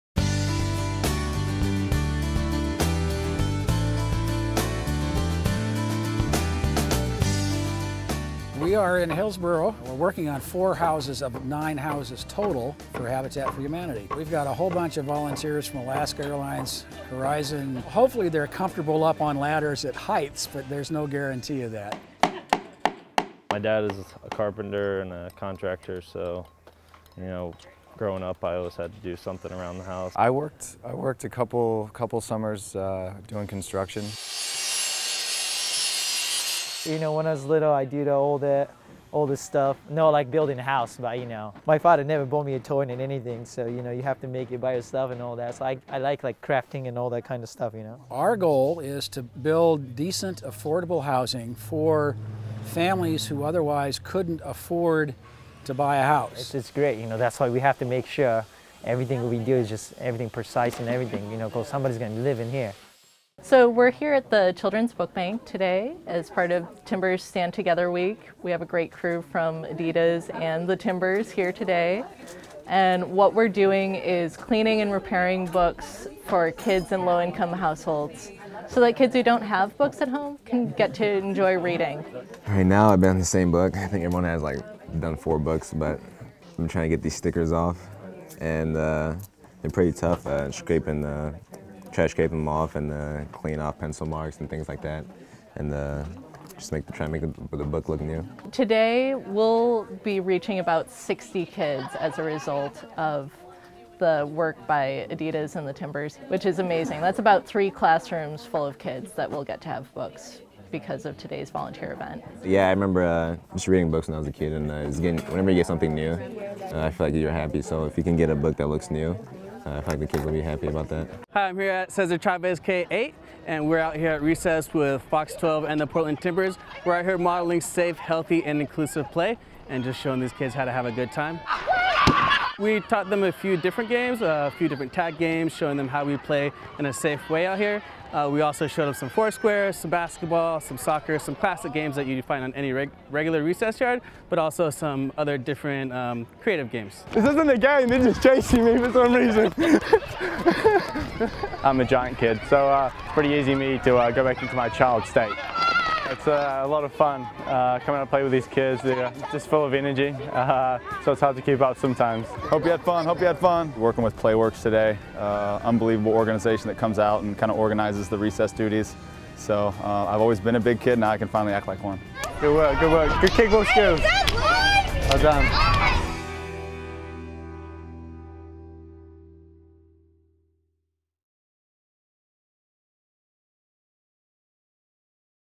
Stand Together Week: Day 2
Bendik, Fucito & Kimura build houses with Alaska Airlines and Habitat for Humanity, Nagbe works with adidas at Children's Book Bank, while Jewsbury & Gleeson play with Fox 12 at Playworks PDX